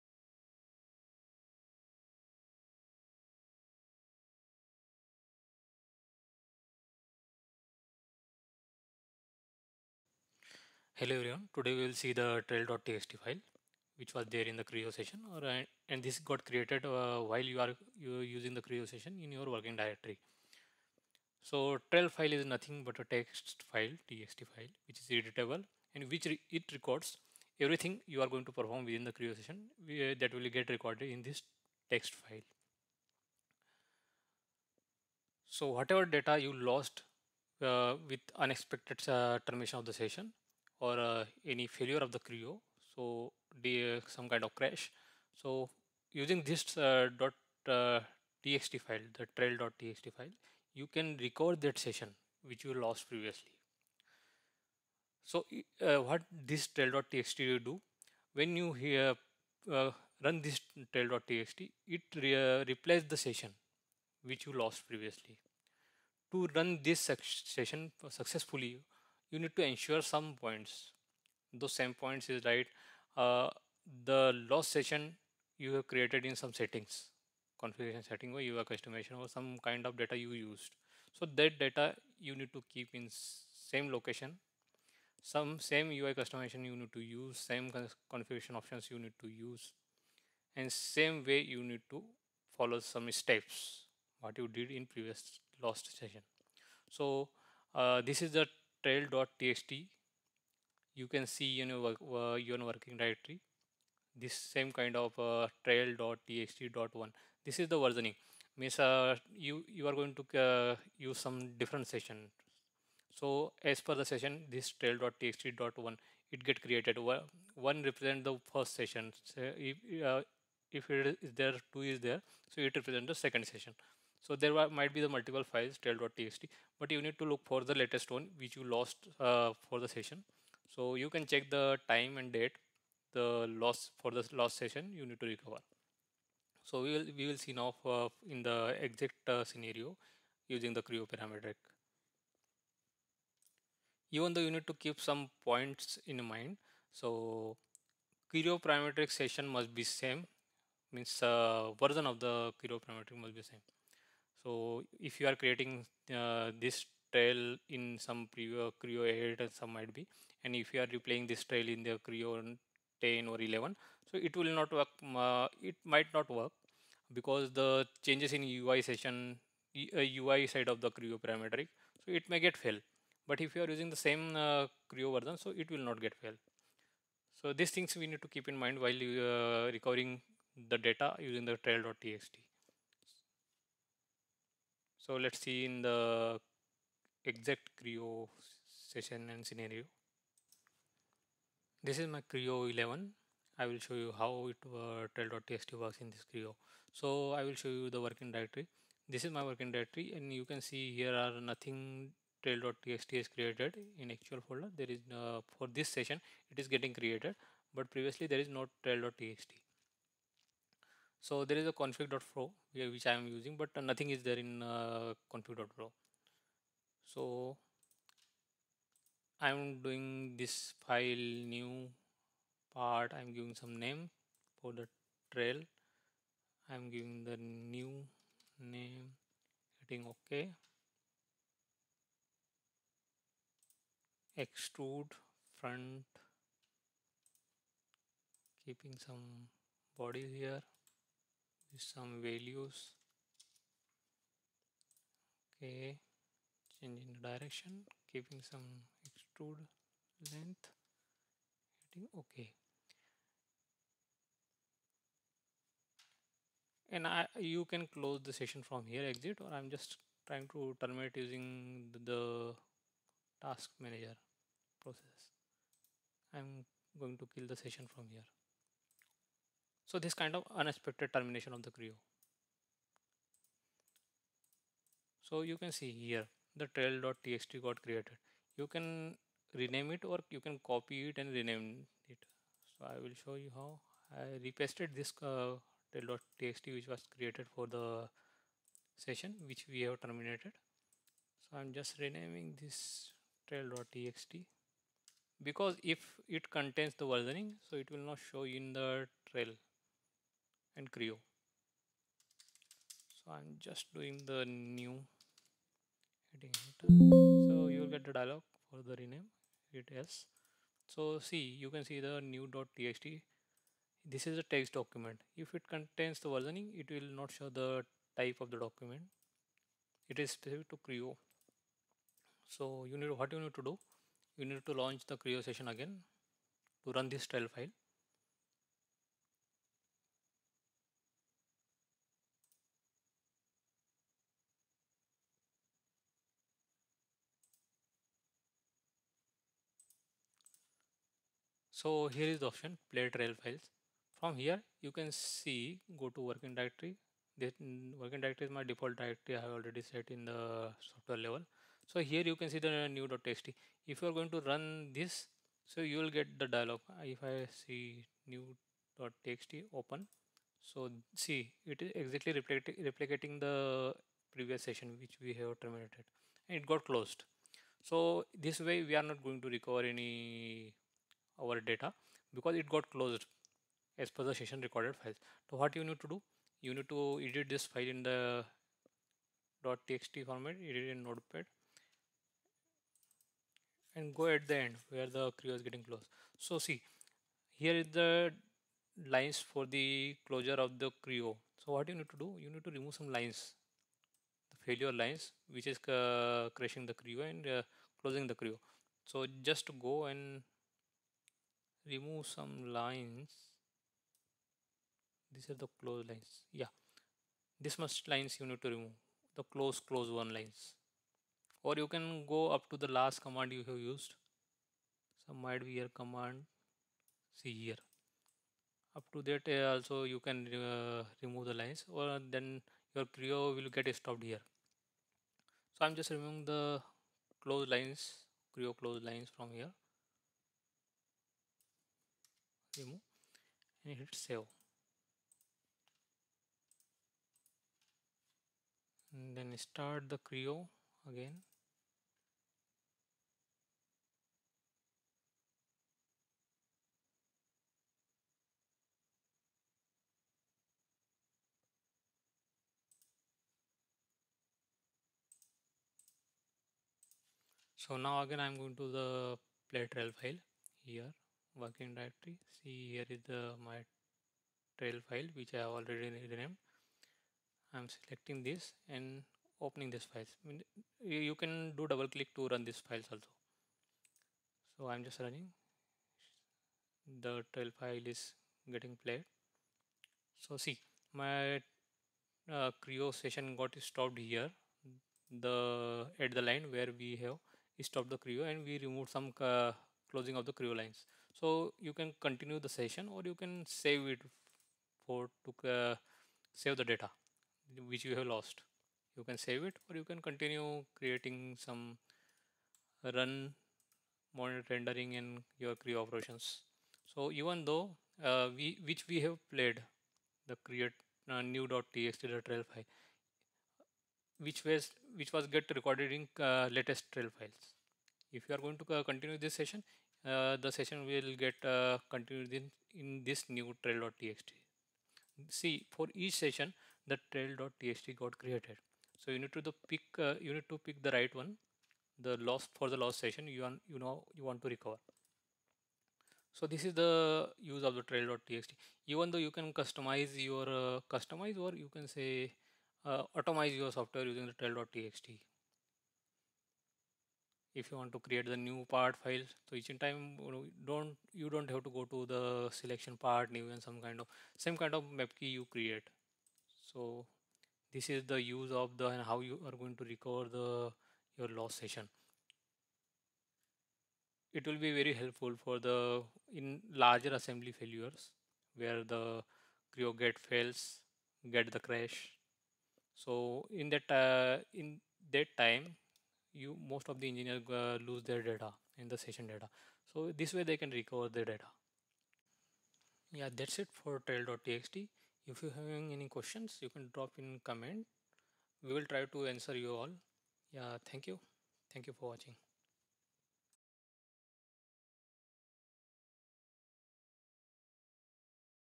Creo Data Recovery After a Crash: Trail.txt secret Explained
In this video, titled "How to Recover Creo Data After a Crash: Trail.txt Secret Explained," we explore a crucial topic for all Creo users: data recovery after unexpected crashes.

Have you ever faced the frustration of losing hours of work due to a system crash? Don't worry! In this tutorial, we unveil the hidden gem of Creo—the Trail.txt file. This powerful tool can help you retrieve lost data and get back on track quickly.

In this video, you will learn:

- What the Trail.txt file is and its role in Creo
- Step-by-step instructions on how to locate and use the Trail.txt file for data recovery
- Best practices to minimize data loss and improve your workflow
- Tips for preventing crashes and ensuring a smoother experience in Creo

Whether you're a beginner or an experienced user, this video will equip you with essential knowledge to safeguard your designs and recover lost work efficiently.